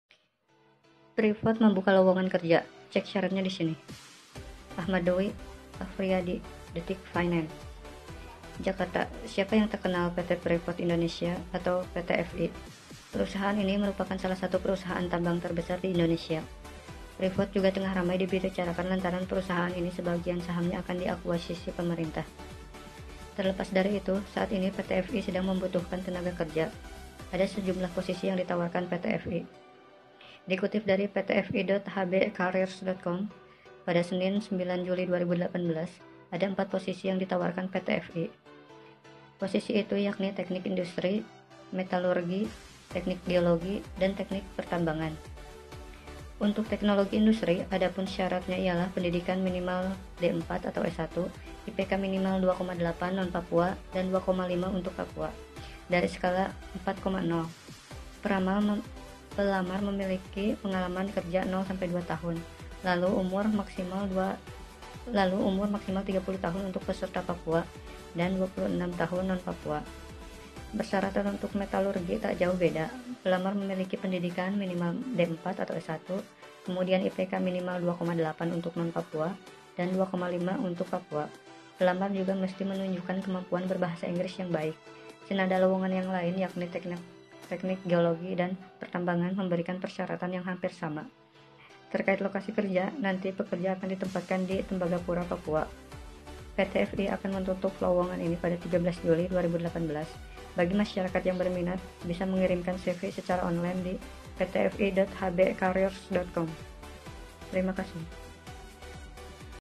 Lowongan Kerja PT Freeport Indonesia 2018
PT Freeport membuka lowongan kerja tanggal 9 Juli 2018 ada 4 lowongan kerja yang dibuka PT pripot syaratnya adalah minimal S1 umur 30 untuk Papua dan 26 untuk non Papua lowongan ini dibuka untuk lokasi Tembagapura Papua lowongan ini ditutup pada 13 Juli 2018 Jadi bagi Anda yang berniat untuk melamar pekerjaan ini silakan membuka Link berikut ini  ptfi.hbcareers.com